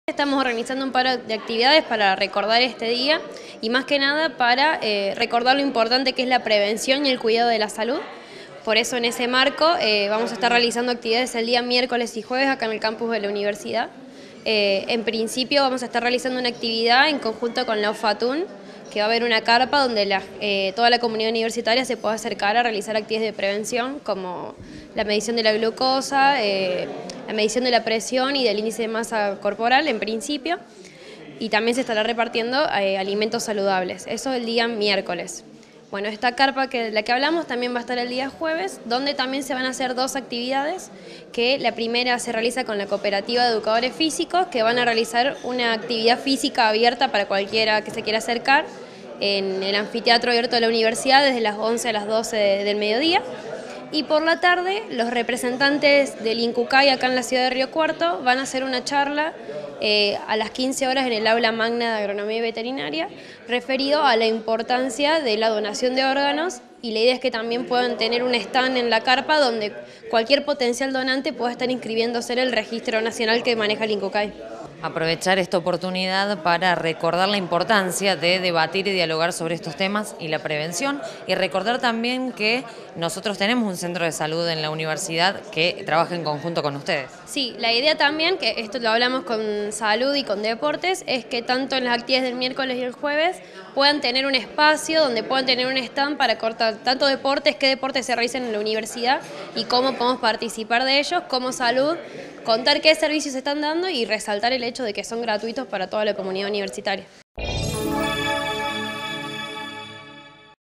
DIA MUNDIAL DE LA SALUD - AGUSTINA AGÜERO
REALIZARÁN ACTIVIDADES DE PREVENCIÓN Y CONTROL DE ENFERMEDADES
Facebook.com/uniriotv
Twitter.com/uniriotv